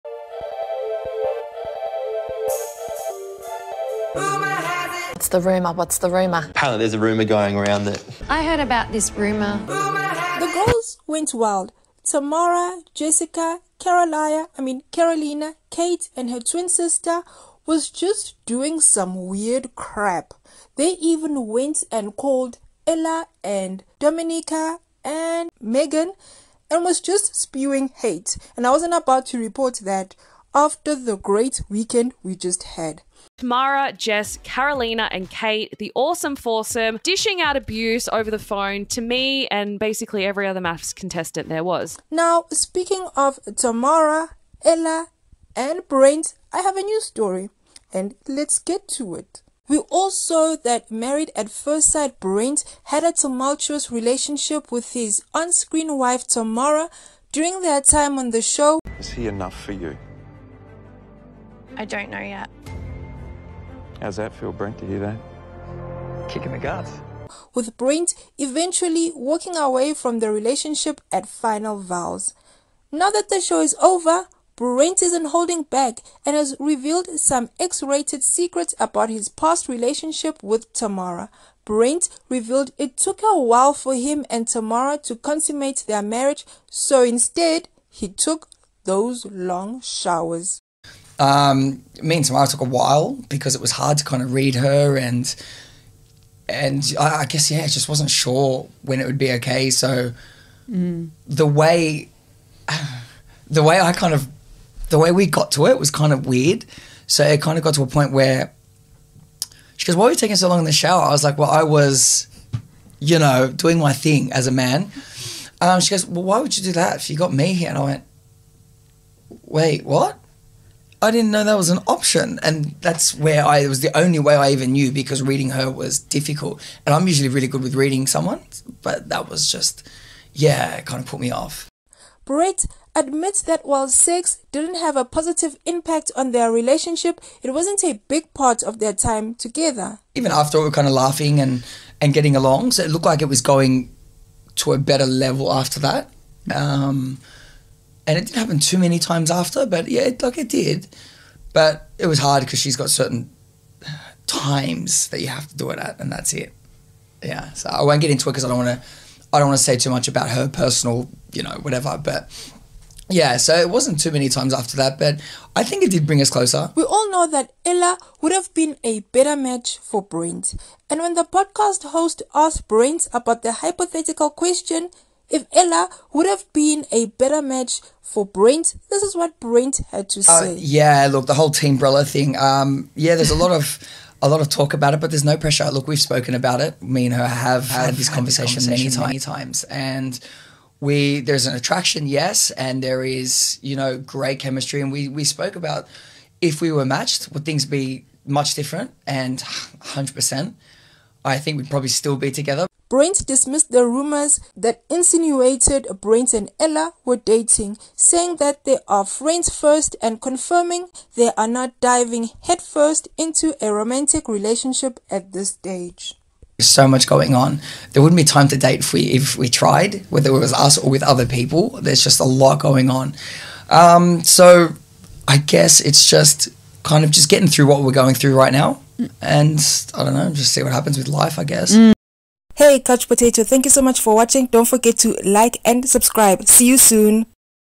MAFS Brent’s steamy X-rated confession
Now that Married at First Sight is over, Brent isnt holding back and has revealed some X-rated secrets about his past relationship with Tamara.